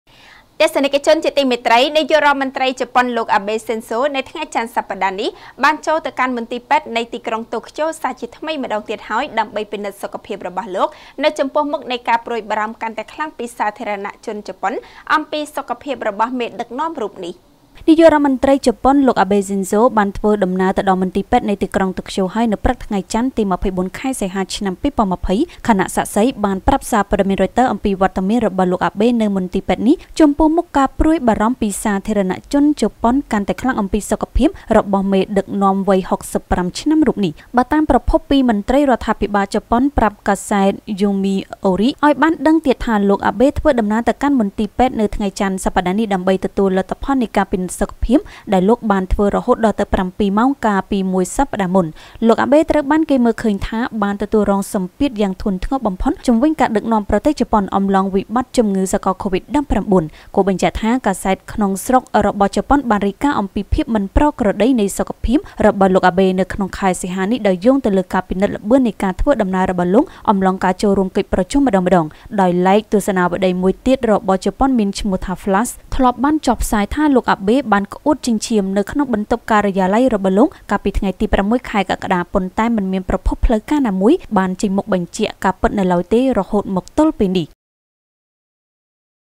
នាយករដ្ឋមន្ត្រីជប៉ុនលោក អាបេ ចូលមន្ទីរពេទ្យជាថ្មី ដើម្បីទទួលលទ្ធផលពិនិត្យសុខភាព.
* រាយការណ៍ព័ត៌មានដោយអ្នកនាង យ៉ង់ សក្គុណា  និងកញ្ញា យឹម ចាន់ស្រីវីន , ទូរទស្សន៍ព័ត៌មាន Fresh News